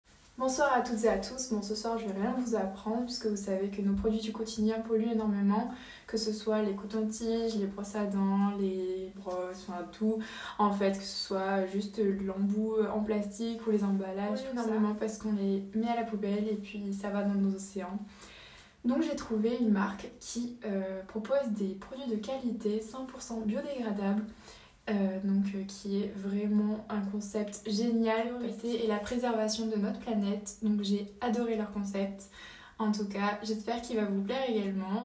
Notre Miss Languedoc-Roussillon vous en apprend davantage sur notre Concept !
Lola Brengues, notre Miss Languedoc-Roussilon 2018, nous en apprend davantage sur la pollution de plastique et sur le concept écologique et novateur de  My Daily Green Care !
Notre Instagram : my_daily_green_care